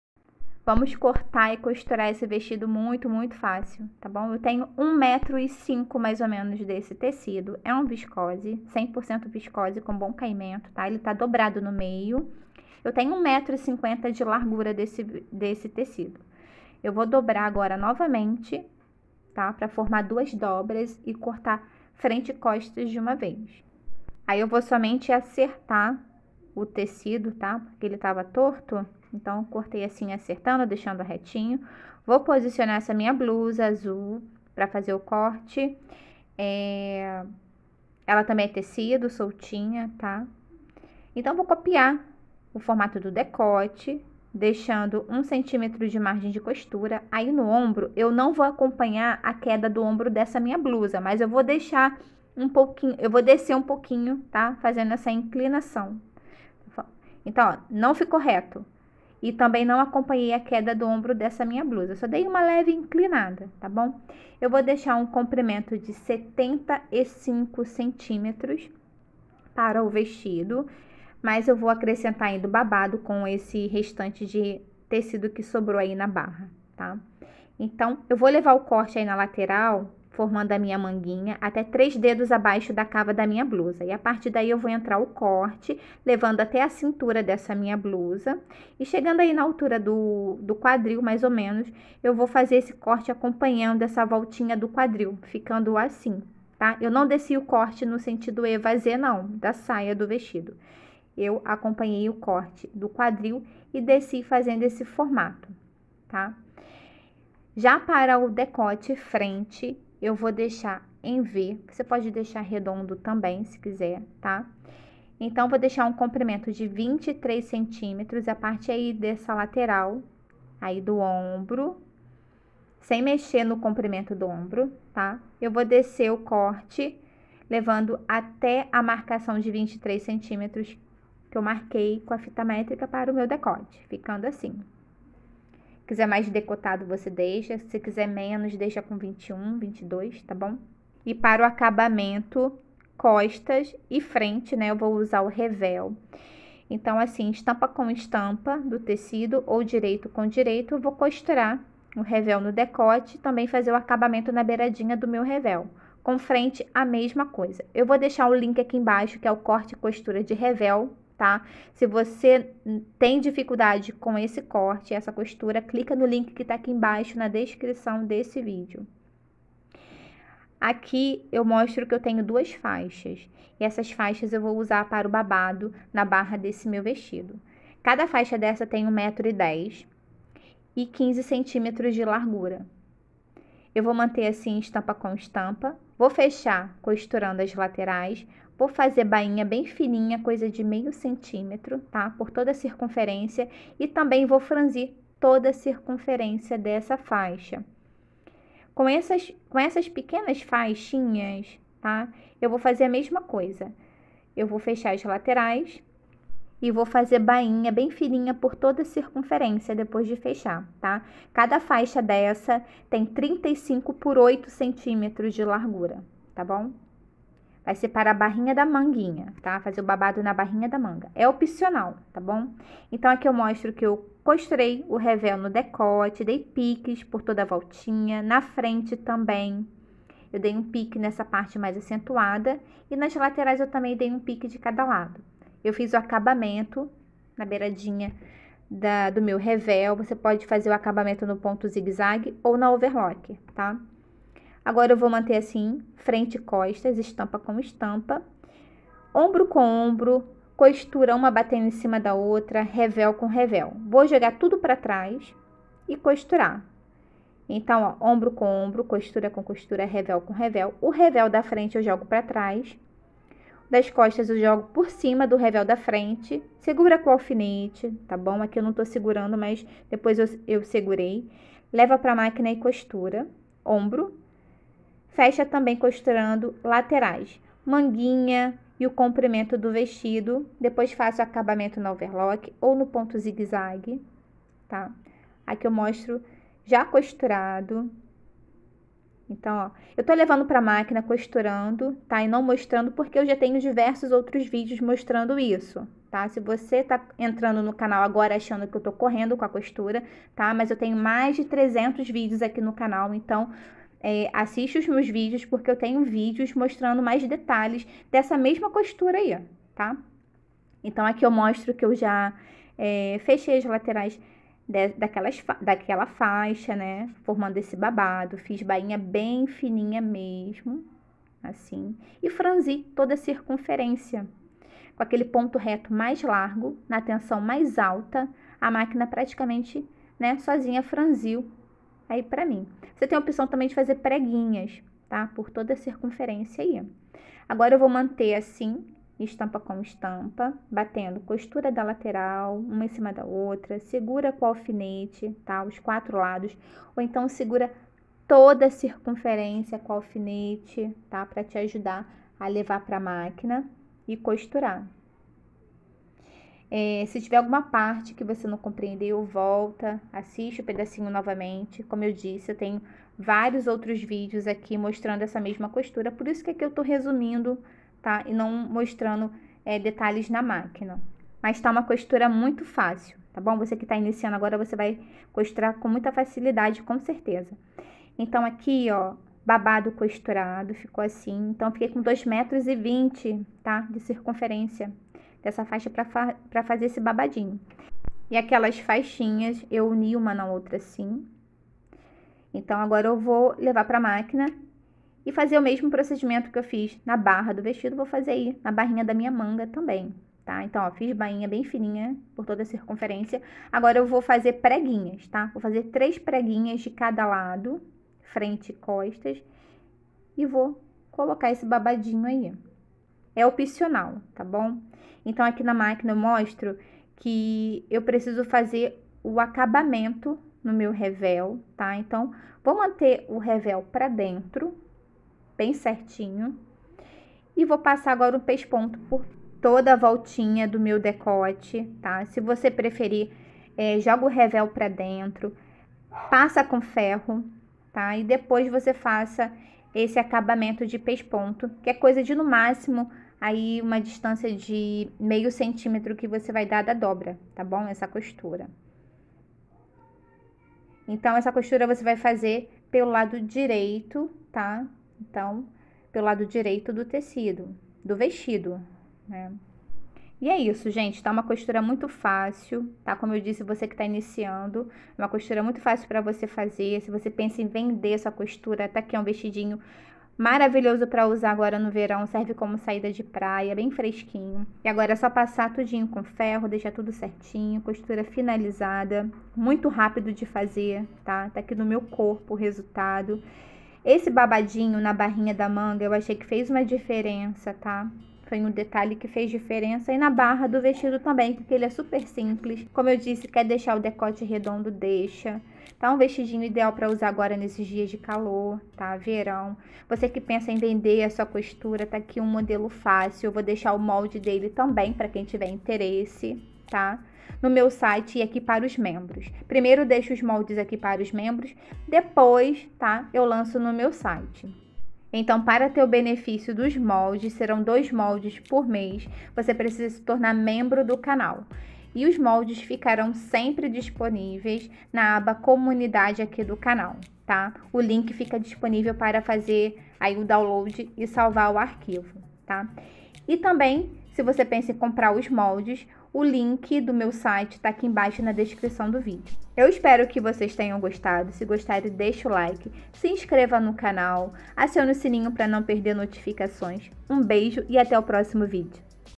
Vestido corte e costura Fácil e sem molde para Iniciantes. DIY Moldes no site, link na Descrição.
Atenção: opção Membro não existe mais! Esse vídeo é repost. Moldes somente no site.

CORTE E COSTURA DE REVEL NO DECOTE Usei Viscose.

Use uma Blusa soltinha, para fazer o corte, assim vc têm suas medidas/ circunferências